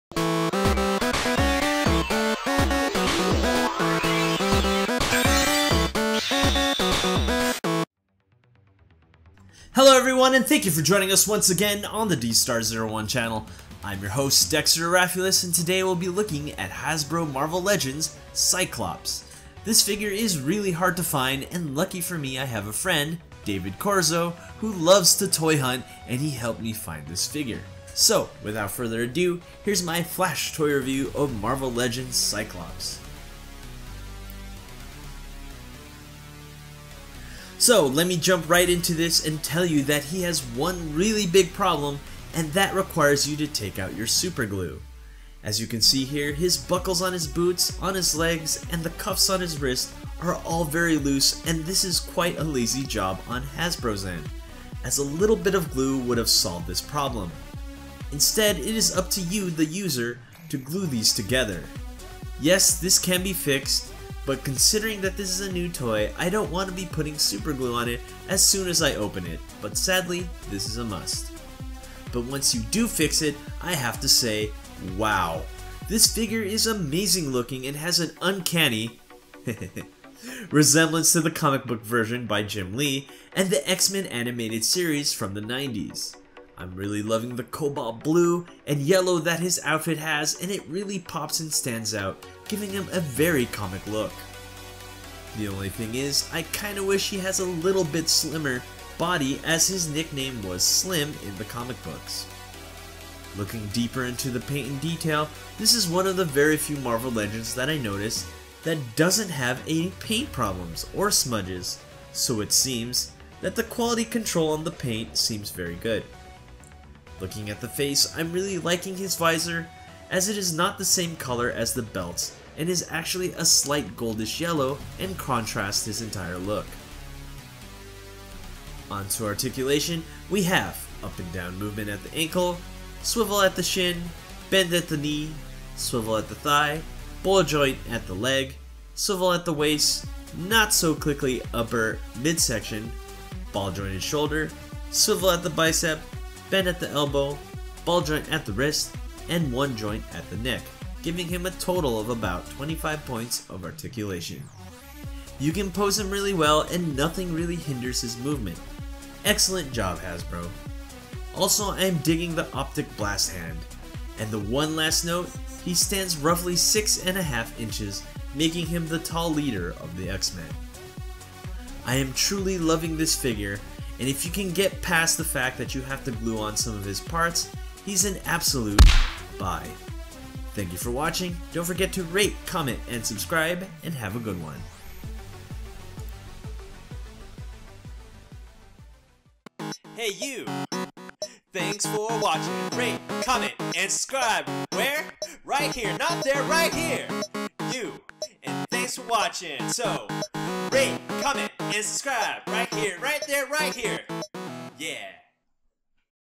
Flash! Toy Review: Marvel Legends Cyclops (X-men series 2017)
For the best selection and best prices on all of the hottest action figures,

Marvel Legends bringing back a classic character, but is he worth the buy? Let's find out!

Written by: Dexter Arafiles
Directed by: Janelle Arafiles
Theme by: Alex Peng

John 15:12-14